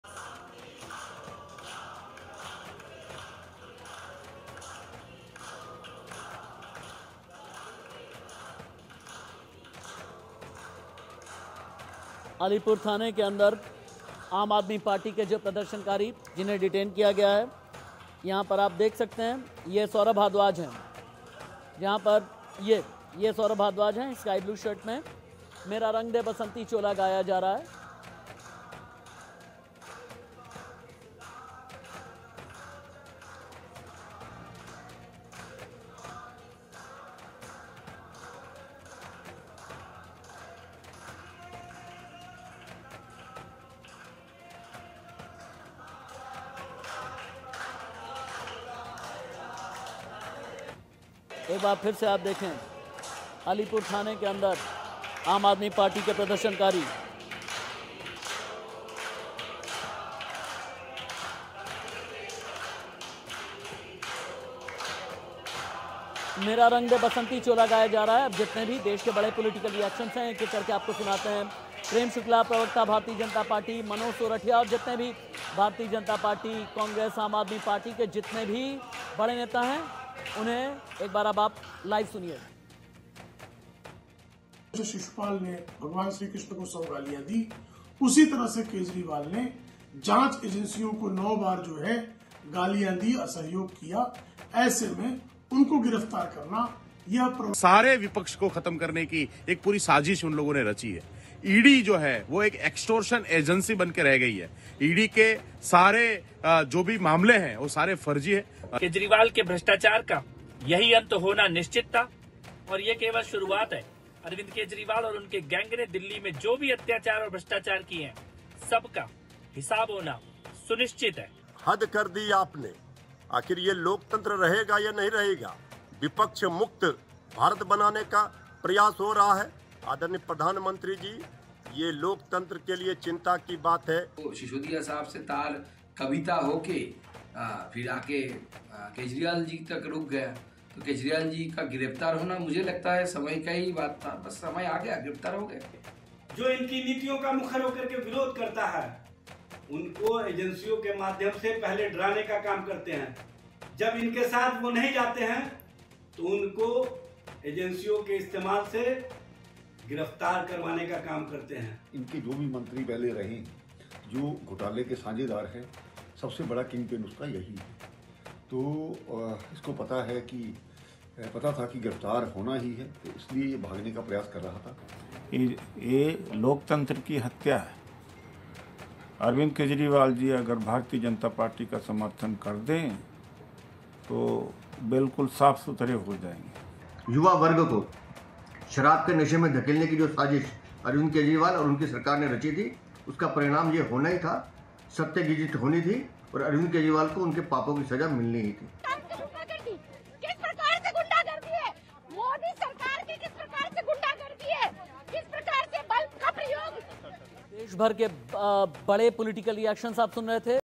Arvind Kejriwal Arrest: अलीपुर थाने के अंदर आम आदमी पार्टी के प्रदर्शनकारी | India News | Delhi CM
अलीपुर थाने के अंदर आम आदमी पार्टी के प्रदर्शनकारी जोरोशोरों से गाना गाते हुए प्रदर्शन कर रहे है , साथ ही आम आदमी पार्टी दफ्तर के आसपास दिल्ली पुलिस ने भारी सुरक्षा के इंतजाम किए हैं।  AAP आने वाले सभी रास्तों पर बैरिकेडिंग लगाए गए हैं। आईटीओ, बहादुर शाह जफर मार्ग और मिंटो रोड से आप दफ्तर पहुंचने वाले रास्तों को बंद किया गया। अरविंद केजरीवाल की गिरफ्तारी के विरोध में आम आदमी पार्टी आज बीजेपी के खिलाफ प्रदर्शन करेगी।